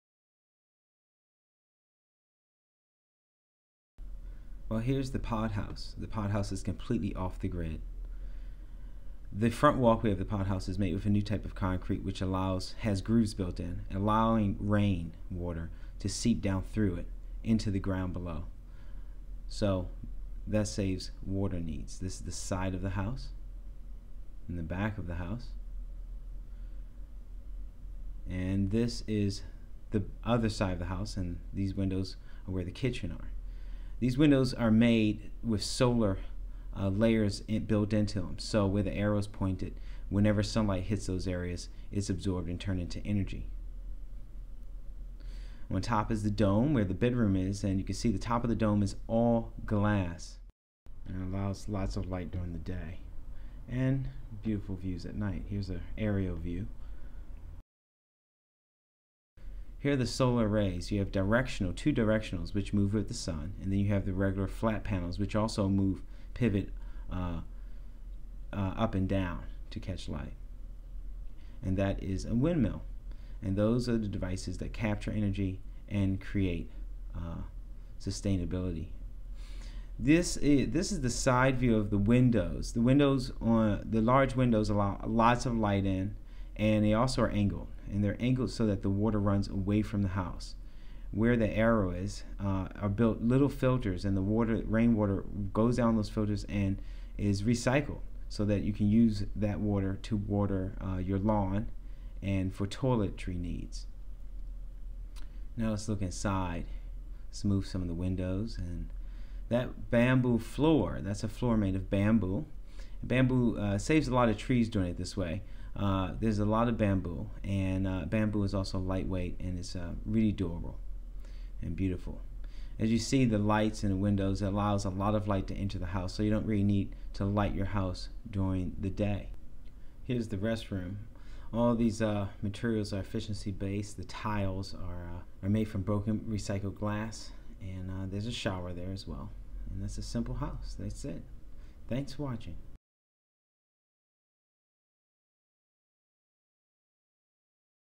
Pod Home Demo
This is the pod home. Which is made with recycled and bio-friendly materials.